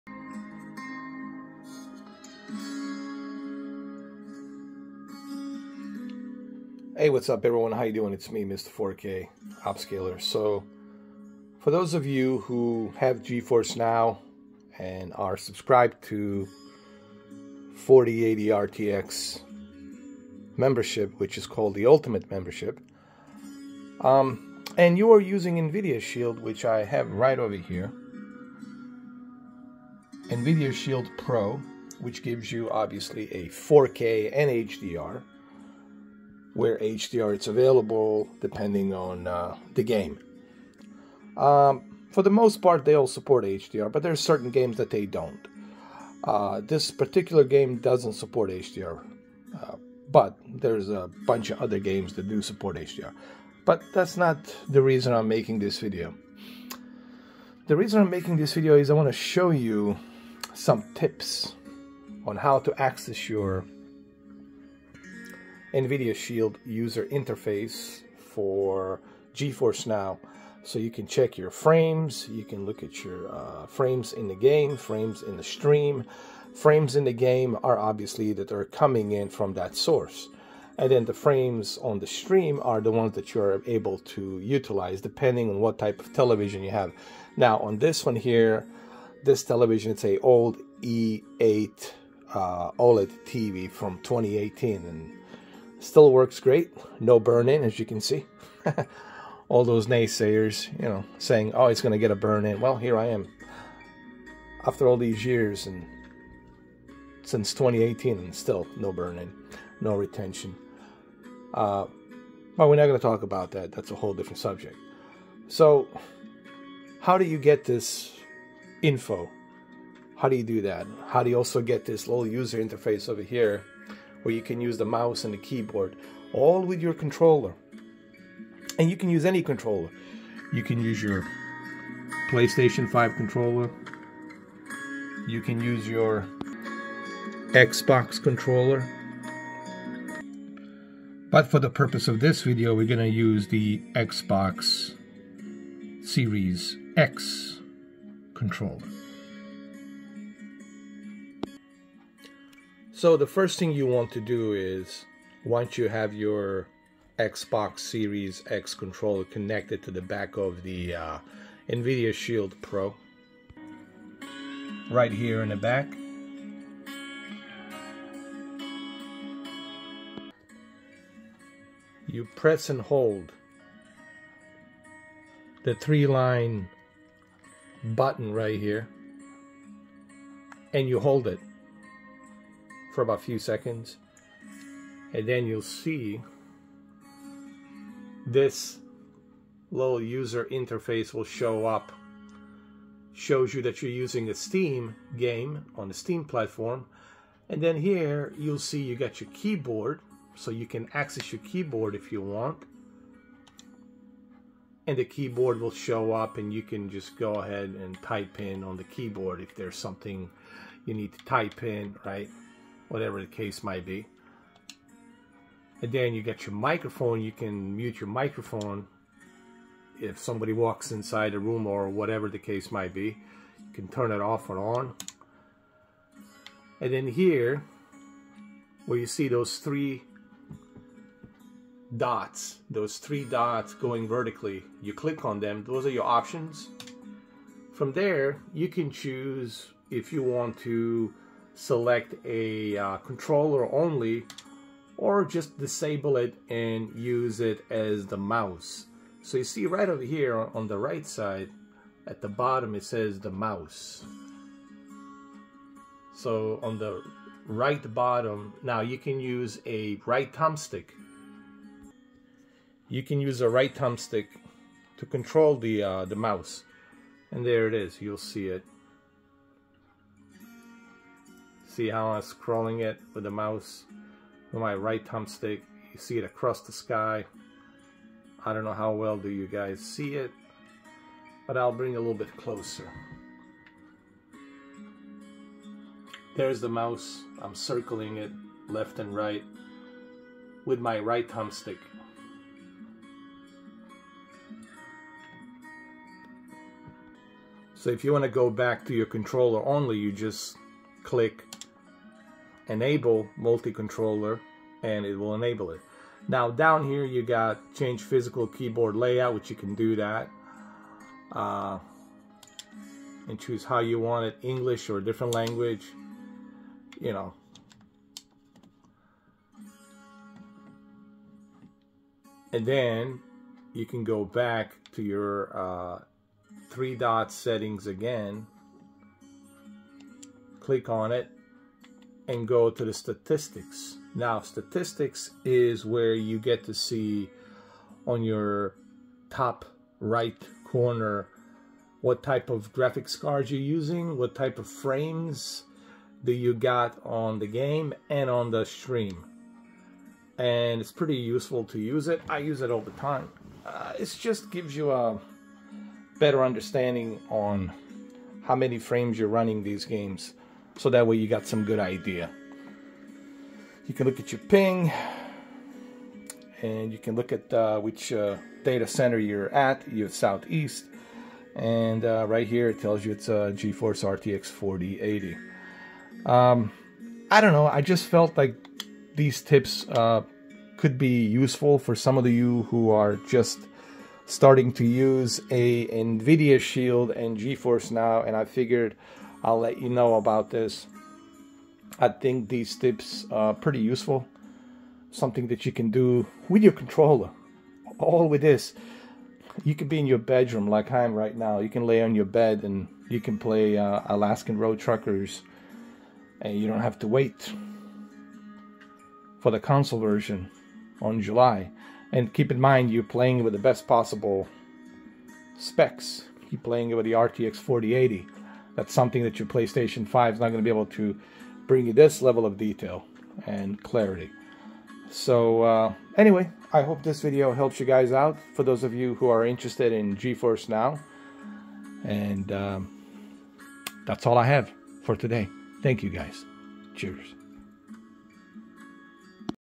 GeForce Now & Useful Tips while using Nvidia Shield Pro
A quick video for those of you , who are thinking about using a Nvidia Shield Pro for your GeForce Now Ultimate Membership RTX 4080 24GB Tier , Here are some useful tips  .